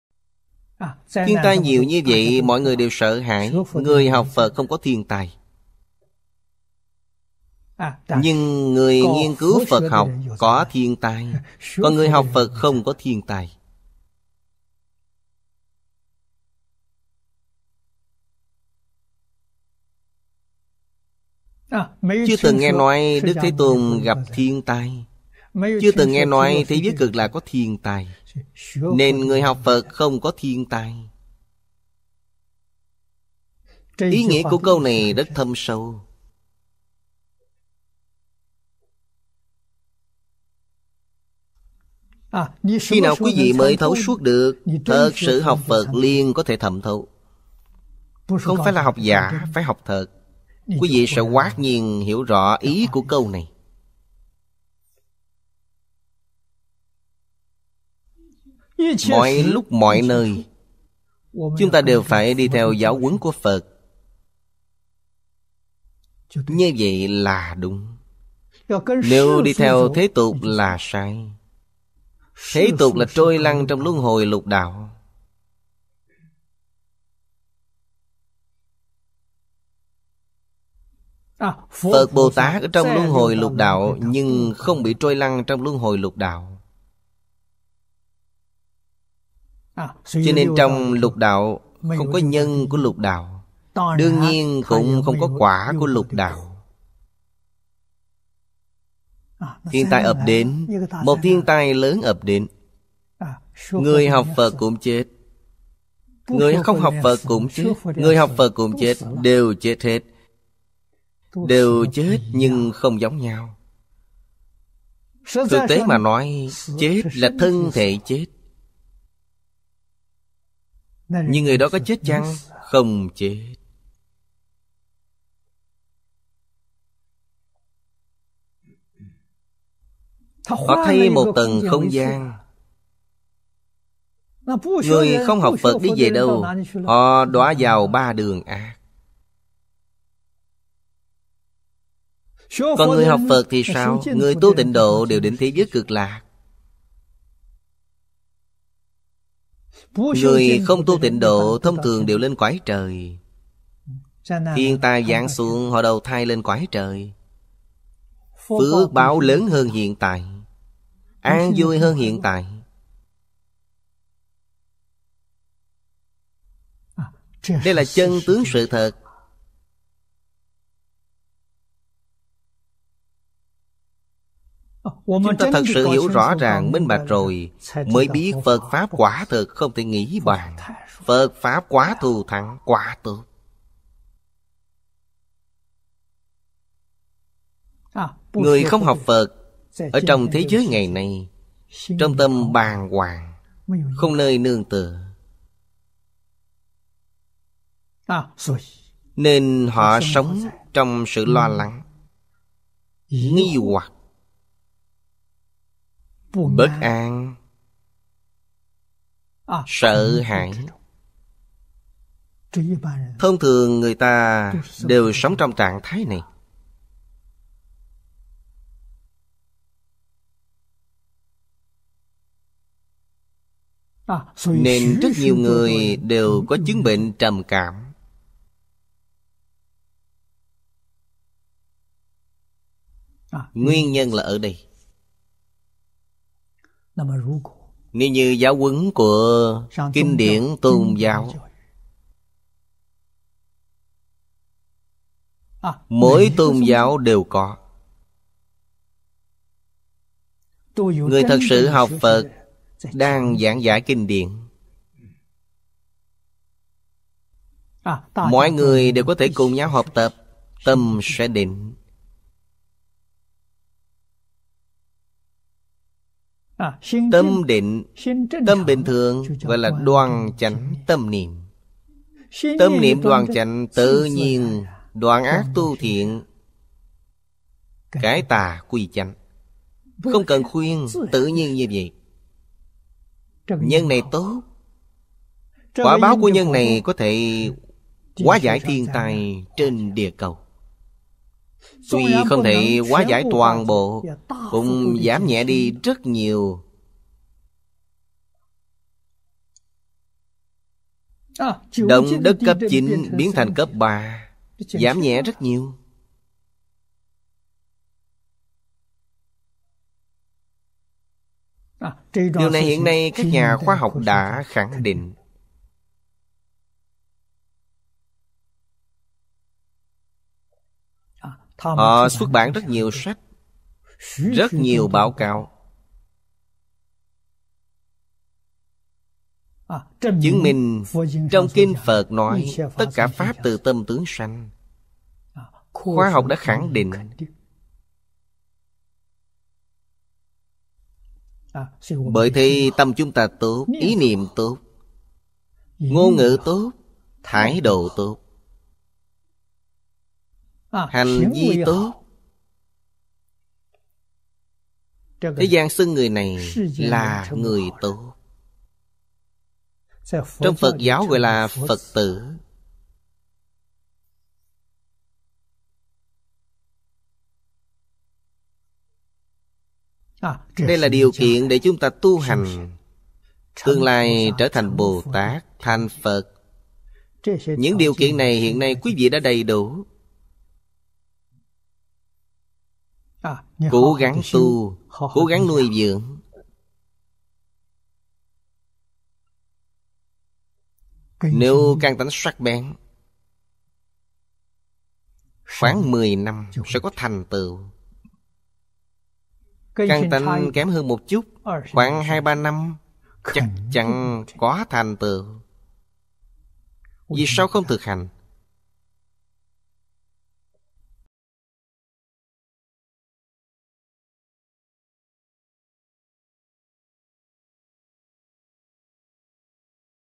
TĐ:2616- Người học Phật không bị thiên tai
Thiên tai nhiều như vậy, mọi người đều sợ hãi. Người học Phật không có thiên tai, nhưng người nghiên cứu Phật học có thiên tai, còn người học Phật không có thiên tai. Chưa từng nghe nói Đức Thế Tôn gặp thiên tai, chưa từng nghe nói thế giới Cực Lạc có thiên tai, nên người học Phật không có thiên tai. Ý nghĩa của câu này rất thâm sâu, khi nào quý vị mới thấu suốt được? Thật sự học Phật liền có thể thẩm thấu, không phải là học giả, phải học thật, quý vị sẽ hoát nhiên hiểu rõ ý của câu này.
      Mọi lúc mọi nơi, chúng ta đều phải đi theo giáo huấn của Phật, như vậy là đúng. Nếu đi theo thế tục là sai, thế tục là trôi lăn trong luân hồi lục đạo. Phật Bồ Tát ở trong luân hồi lục đạo, nhưng không bị trôi lăn trong luân hồi lục đạo, cho nên trong lục đạo không có nhân của lục đạo, đương nhiên cũng không có quả của lục đạo. Thiên tai ập đến, một thiên tai lớn ập đến, người học Phật cũng chết, người không học Phật cũng chết, người học Phật cũng chết, đều chết hết, đều chết nhưng không giống nhau! Thực tế mà nói, chết là thân thể chết, nhưng người đó có chết chăng? Không chết! Họ thay một tầng không gian. Người không học Phật đi về đâu? Họ đọa vào ba đường ác, còn người học Phật thì sao? Người tu Tịnh độ đều đến thế giới Cực Lạc, người không tu Tịnh độ thông thường đều lên cõi trời. Thiên tai giáng xuống, họ đầu thai lên cõi trời, phước báo lớn hơn hiện tại, an vui hơn hiện tại, đây là chân tướng sự thật. Chúng ta thật sự hiểu rõ ràng minh bạch rồi, mới biết Phật pháp quả thật không thể nghĩ bàn, Phật pháp quá thù thắng, quá tốt. Người không học Phật ở trong thế giới ngày nay, trong tâm bàng hoàng, không nơi nương tựa, nên họ sống trong sự lo lắng, nghi hoặc, bất an, sợ hãi. Thông thường người ta đều sống trong trạng thái này, nên rất nhiều người đều có chứng bệnh trầm cảm, nguyên nhân là ở đây.
      Nếu như giáo huấn của kinh điển Tôn giáo, mỗi tôn giáo đều có, người thật sự học Phật đang giảng giải kinh điển. Mọi người đều có thể cùng nhau học tập, tâm sẽ định. Tâm định, tâm bình thường, gọi là đoan chánh tâm niệm. Tâm niệm đoan chánh tự nhiên đoạn ác tu thiện, cải tà quy chánh, không cần khuyên, tự nhiên như vậy. Nhân này tốt, quả báo của nhân này có thể hóa giải thiên tai trên địa cầu. Tuy không thể hóa giải toàn bộ, cũng giảm nhẹ đi rất nhiều, động đất cấp chín biến thành cấp ba, giảm nhẹ rất nhiều.
      Điều này hiện nay các nhà khoa học đã khẳng định, họ xuất bản rất nhiều sách, rất nhiều báo cáo, chứng minh trong kinh Đức Phật nói: “tất cả pháp từ tâm tưởng sanh”, khoa học đã khẳng định. Bởi thế tâm chúng ta tốt, ý niệm tốt, ngôn ngữ tốt, thái độ tốt, hành vi tốt, thế gian xưng người này là người tốt, trong Phật giáo gọi là Phật tử. Đây là điều kiện để chúng ta tu hành tương lai trở thành Bồ Tát, thành Phật. Những điều kiện này hiện nay quý vị đã đầy đủ, cố gắng tu, cố giắng nuôi dưỡng. Nếu căn tánh sắc bén khoảng 10 năm sẽ có thành tựu, căn tánh kém hơn một chút khoảng hai ba mươi năm chắc chắn có thành tựu, vì sao không thực hành?